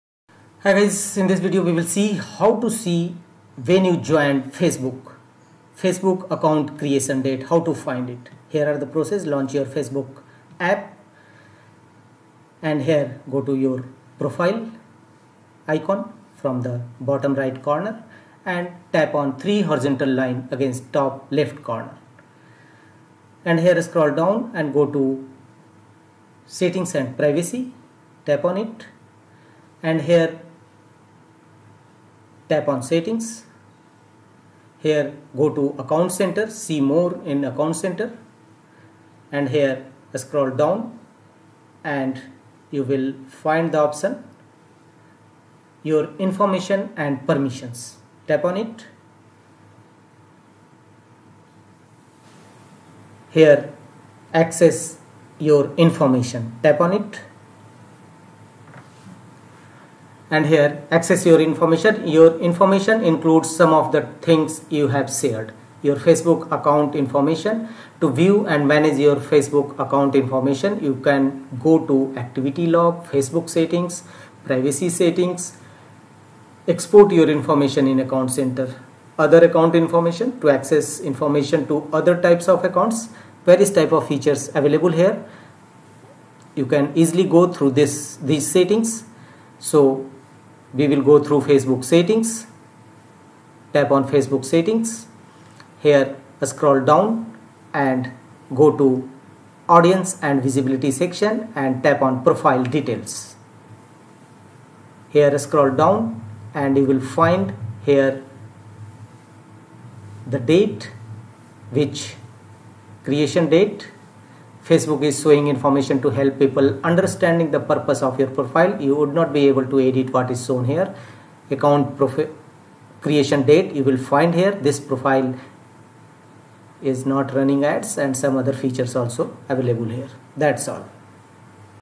When Did You Join Facebook? Here’s How to Check
Want to know exactly when you joined Facebook? In this quick tutorial, I’ll show you how to find your Facebook account creation date using both the Facebook app and desktop browser.

Works on: Android & iPhone (Mobile App), Desktop/Laptop (Browser Version)

In this video, you’ll learn:

Where to find your join date on Facebook

How to access your profile info and activity log

A quick method to see your Facebook history